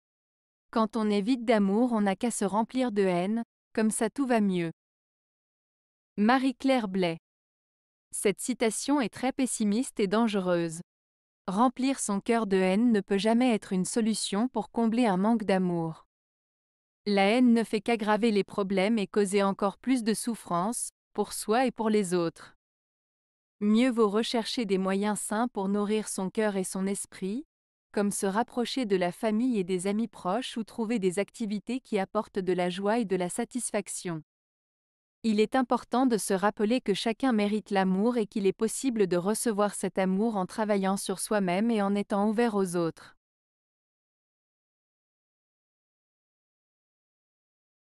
Quand on est vide d'amour on n'a qu'à se remplir de haine, comme ça tout va mieux.
Cette citation est très pessimiste et dangereuse. Remplir son cœur de haine ne peut jamais être une solution pour combler un manque d'amour. La haine ne fait qu'aggraver les problèmes et causer encore plus de souffrance, pour soi et pour les autres. Mieux vaut rechercher des moyens sains pour nourrir son cœur et son esprit, comme se rapprocher de la famille et des amis proches ou trouver des activités qui apportent de la joie et de la satisfaction. Il est important de se rappeler que chacun mérite l'amour et qu'il est possible de recevoir cet amour en travaillant sur soi-même et en étant ouvert aux autres.